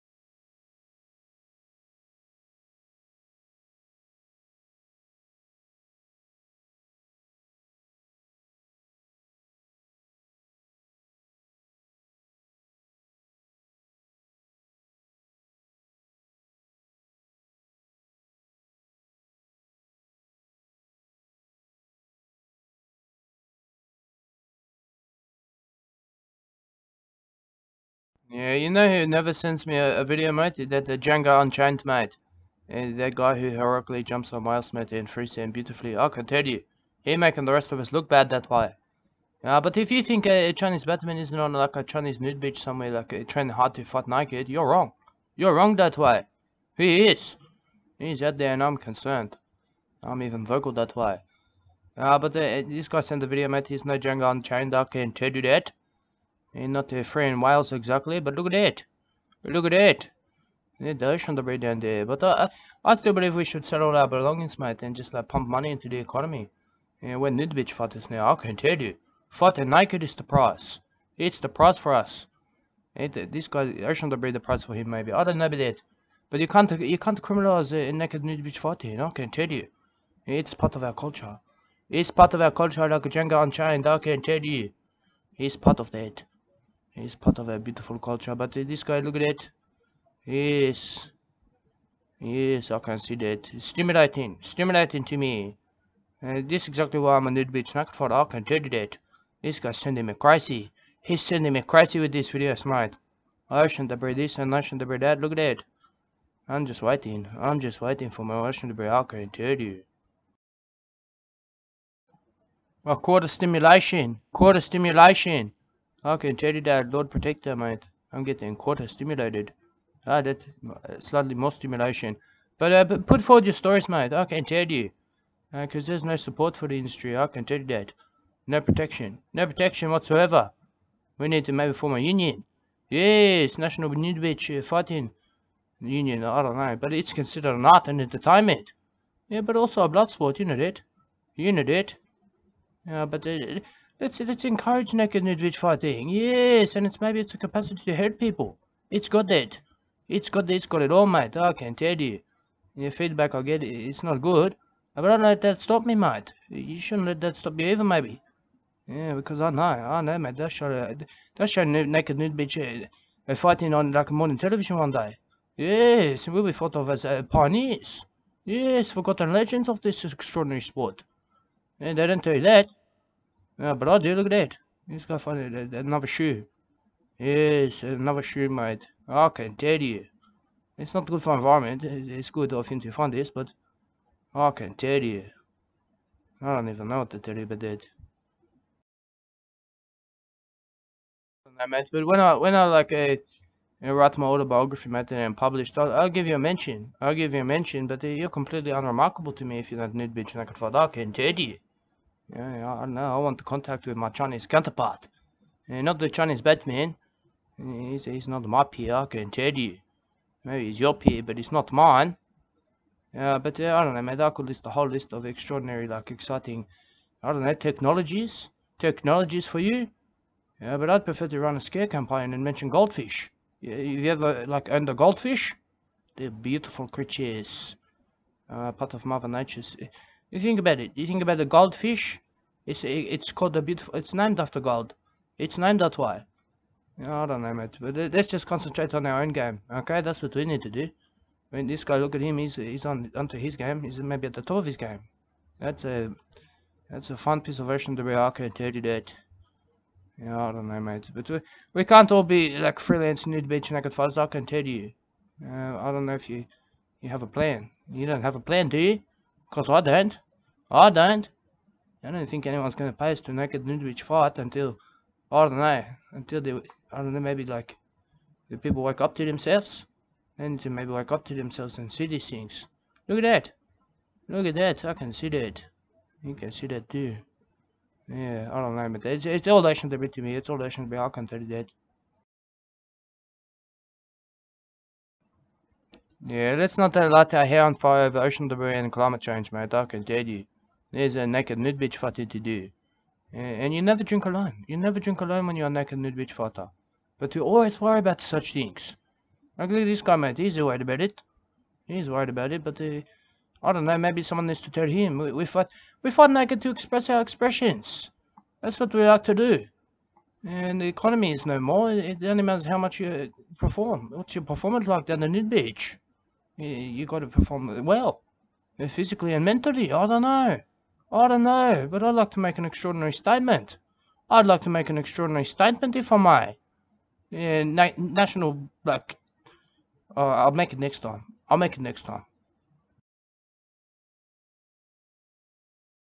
IF YOU THINK THE CHINESE BATMAN ISNT ON A NUDE BEACH SOMEWHERE TRAINING TO FIGHT NAKED YOUR WRONG!
BUSHTICK MICK IS CONCERNED...FIGHTING NAKED IS PART OF OUR CULTURE...PUT FORWARD YOUR STORYS! LETS FORM A UNION! FIGHTING NAKED IS ART & ENTERTAINMENT BUT ALSO A BLOOD SPORT! WE NEED TO ENCOURAGE IT...THE FEED BACK ISNT GOOD BUT WE CANT LET IT STOP US BECAUSE WE ARE PIONEERS! BUT LETS NOT LIGHT OUR HAIR ON FIRE...WE FIGHT NAKED TO EXPRESS OUR EXPRESSIONS...LIKE VIOLENT CHIMPANZEES ON HEAT...